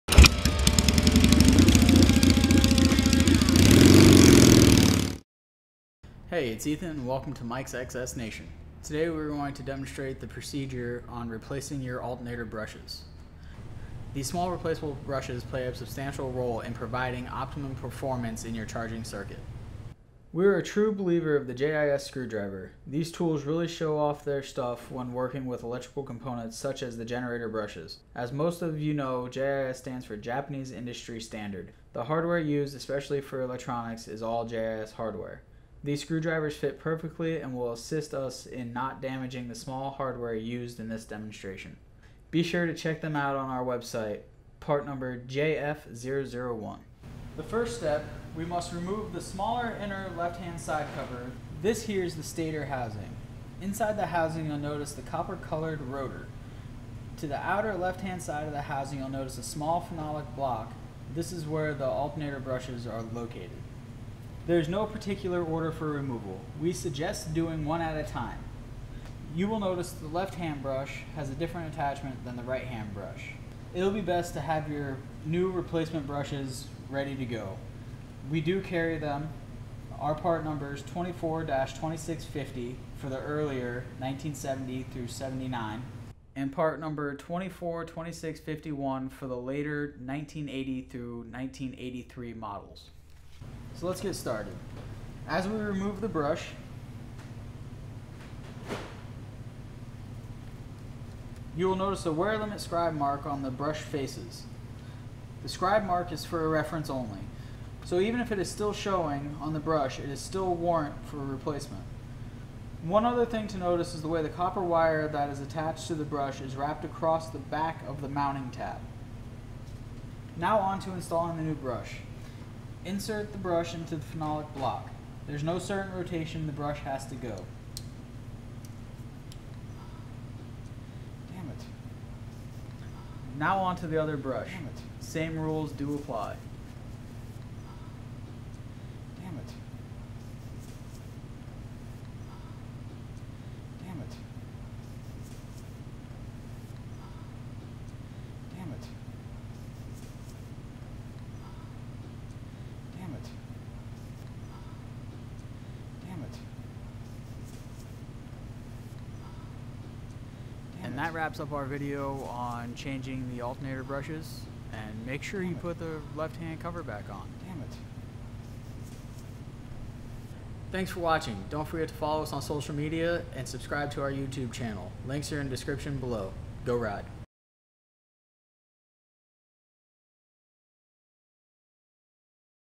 Mikes XS - Replacing Yamaha XS650 Alternator Brushes
Mikes XS : This video will walk you through the proper method of replacing your alternator brushes on a Yamaha XS650 charging system. We provide the parts and tools needed to complete this DIY job!